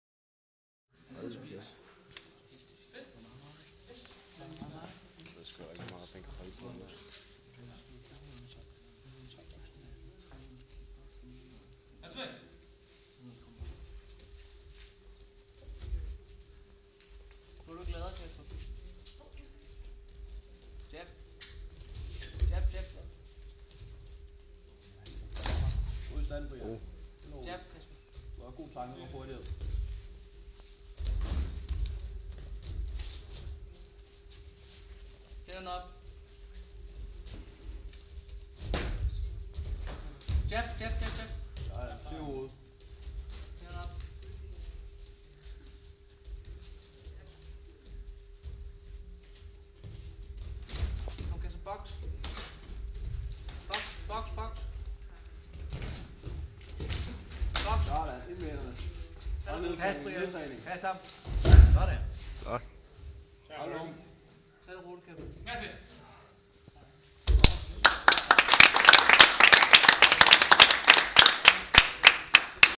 Fight Club vol. VI: Brian vs. Casper
Brian and Casper have a rematch from last time they fought. Nice match..
Unfortunately Casper hurt his knee in the fight, and that was the last fight of the day for him.